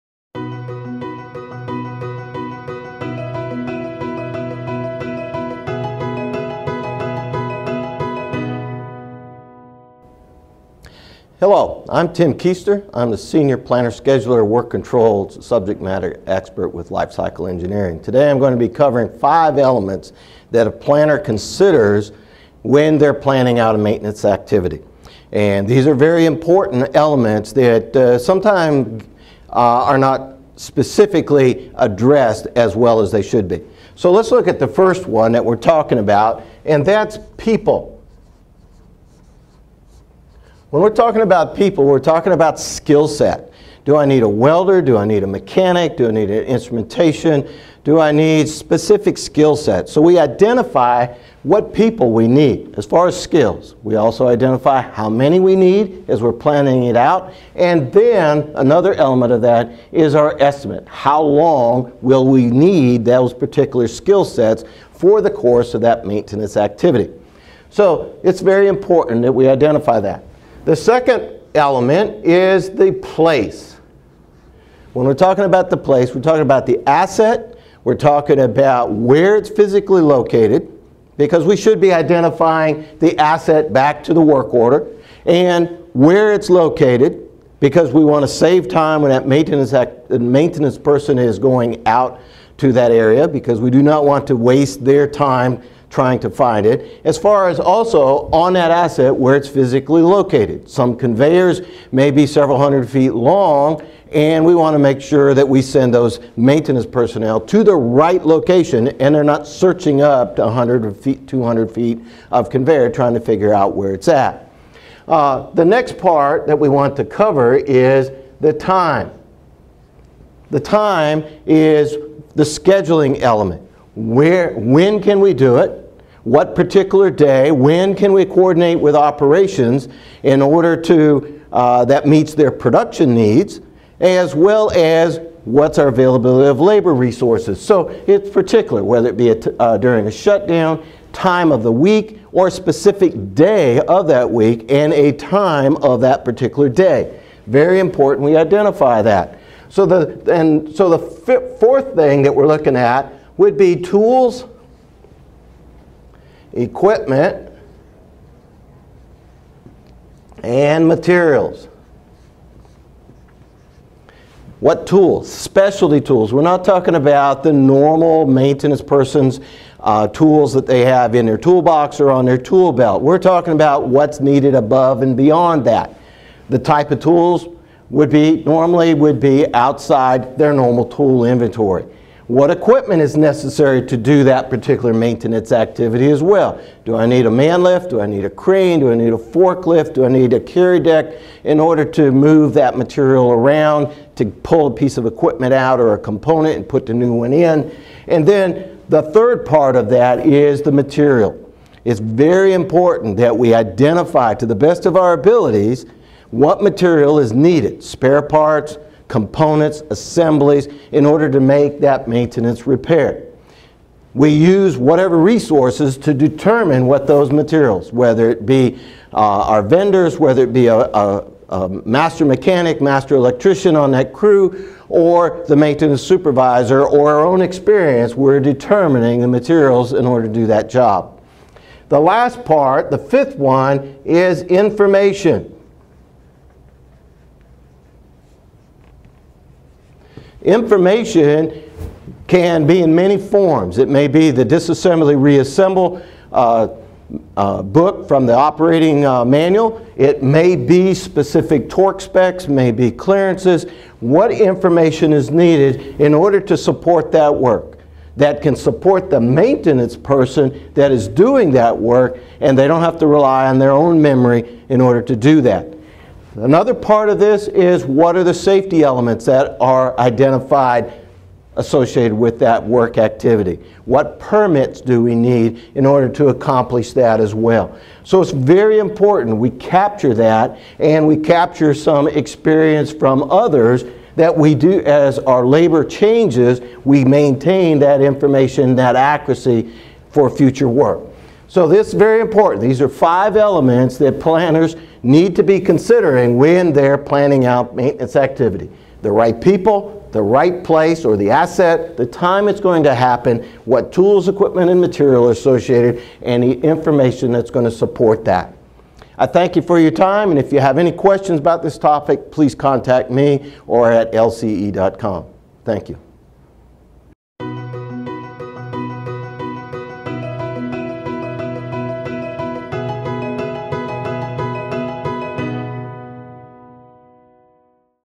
Maintenance Work Planning: 5 Elements to Consider
Tim Kister, Senior Planning and Scheduling SME with Life Cycle Engineering, explains the 5 elements of work planning that planners frequently give limited attention and detail.